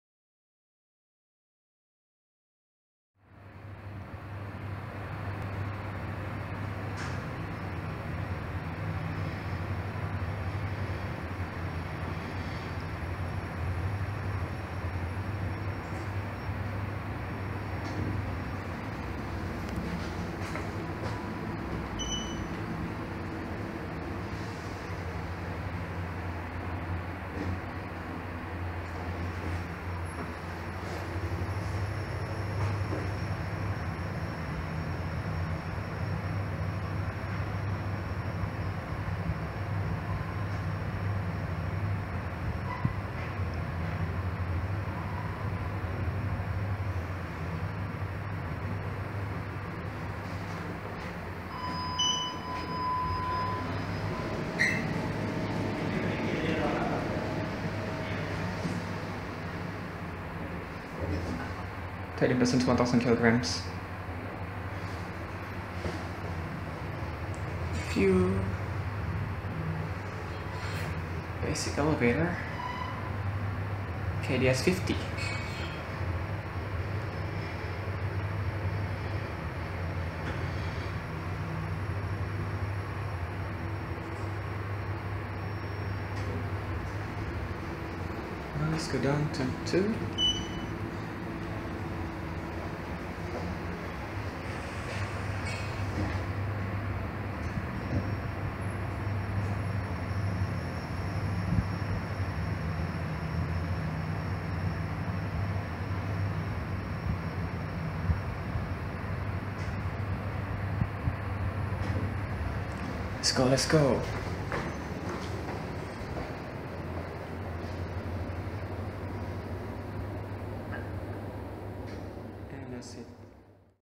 KONE MonoSpace Elevator With KDS 50 Fixtures
Please note, the reason why I filmed this from the corner is try to avoid the security camera inside.

Capacity: 13 Persons/1000kg
Floors served: B, L, 1-3, 5, R (7 floors)
Year installed: 2015
Model / fixtures: MonoSpace 3000 MRL / White KDS 50
History: Original

Fear rating: 0 / 5

Camera: Samsung Galaxy J5
Taken date: XX